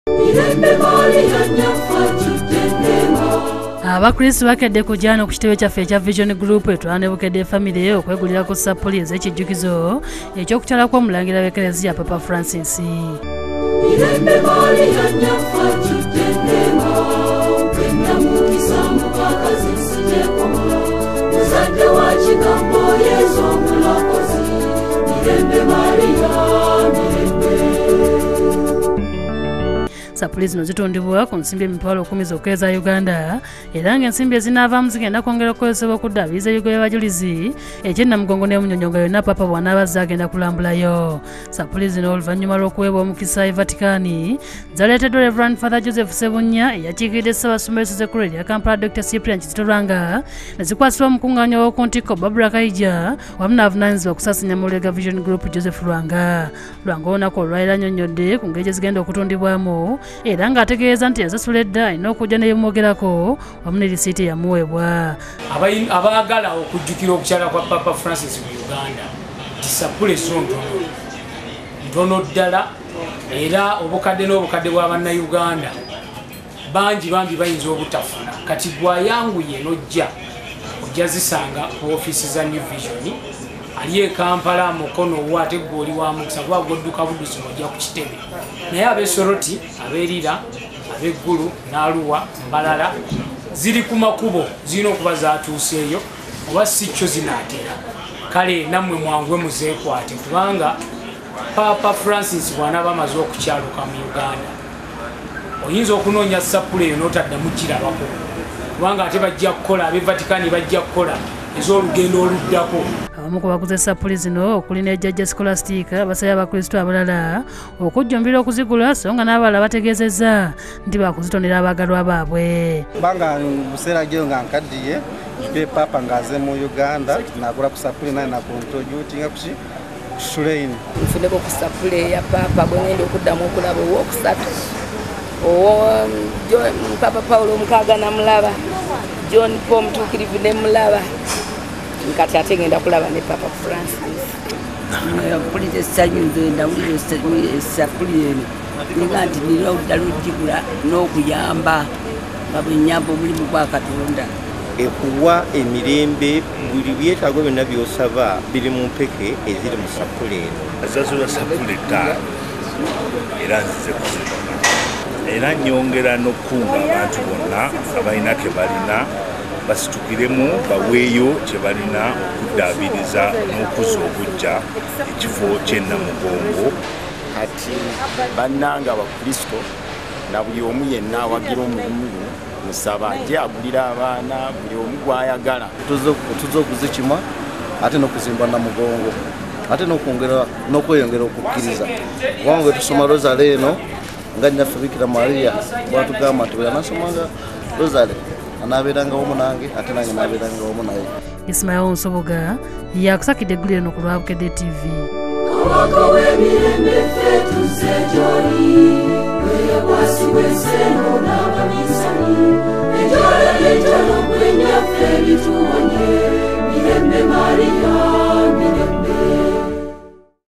Bannayuganda bajjumbidde sapule za Paapa
Abantu basabiddwa okwongera okuzettanira